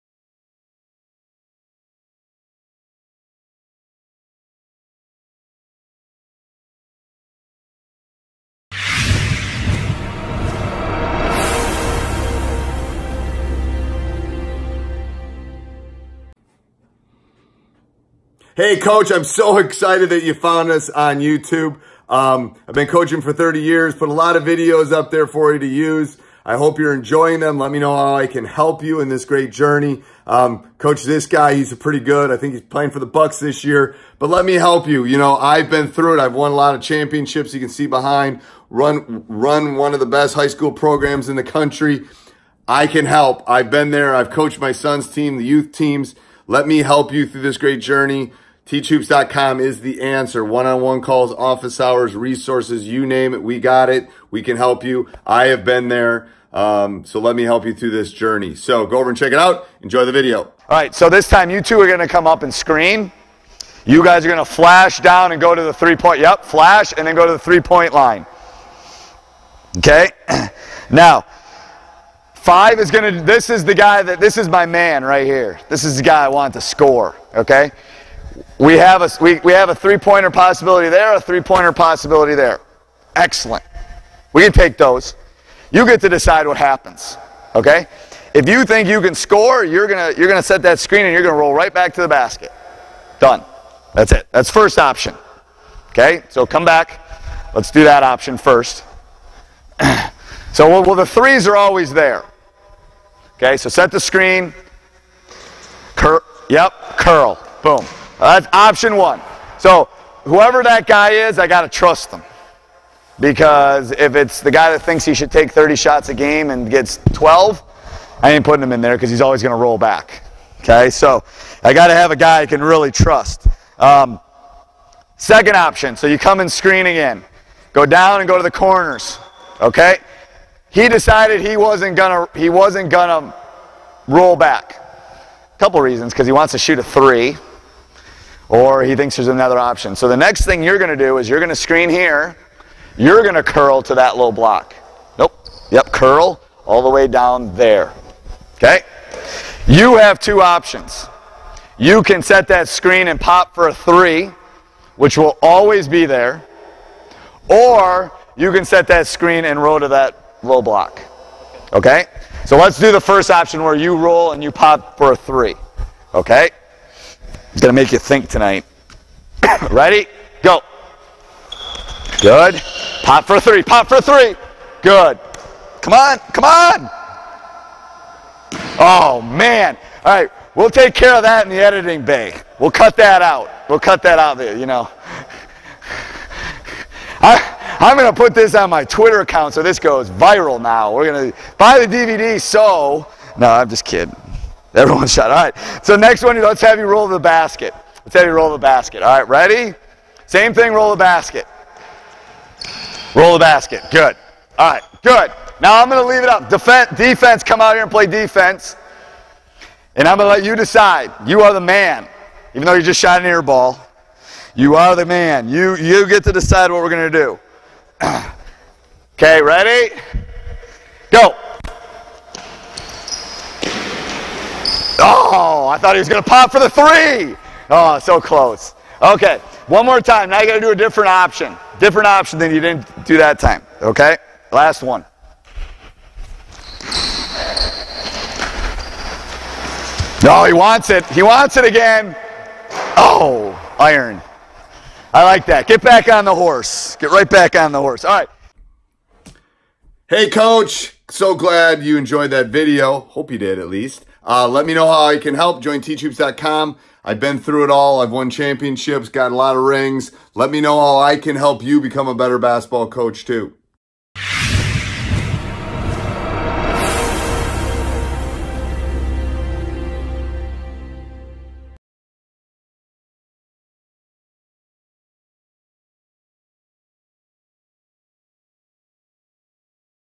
Pick & Flash to Hoop (Box Set): Basketball Inbounds Plays
where everyone can become a better coach!

Another variation out of the box set, Coach shows you a play that has few different tweaks in it & you can decide which one you like the most to implement in your system.